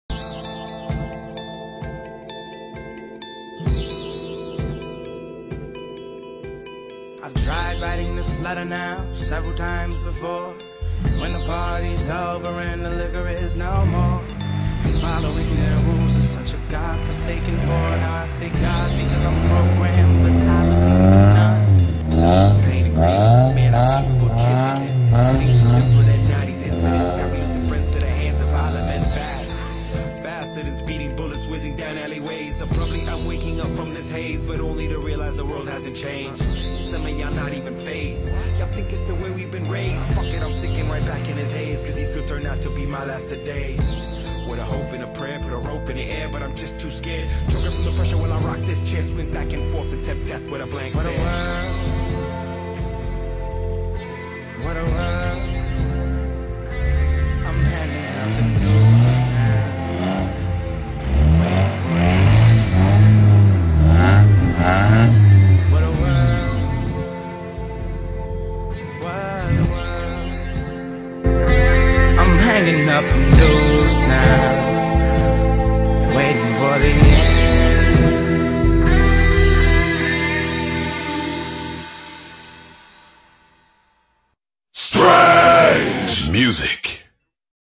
The snow day.
First video of 2019. This is what happens on the Western Slope after a day's work, snow drifts. We take the 94 Civic EX out for some fun to test out the quieter exhaust.